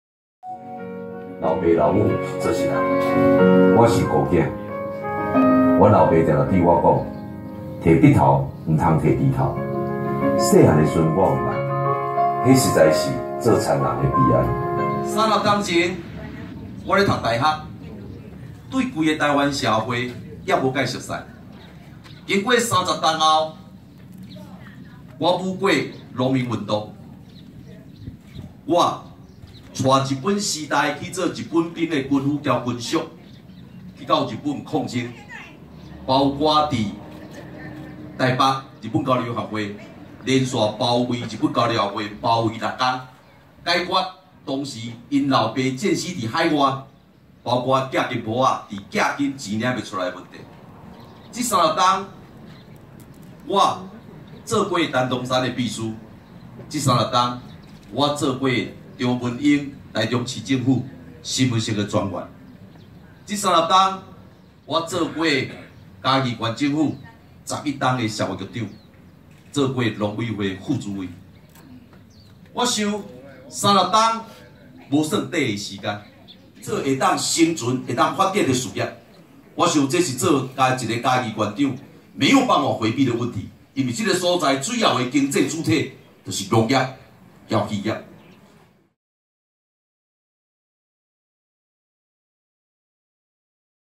拚嘉義縣長初選 翁章梁首支廣告上線
民進黨嘉義縣長黨內初選白熱化，參選人前農委會副主委翁章梁今天在義竹鄉傳芳村老家發表首支競選廣告CF，影片以「嘉義好子弟」為主軸，貫穿從學生運動、地方深耕到中央歷練的歷程，請民眾在初選民調支持他。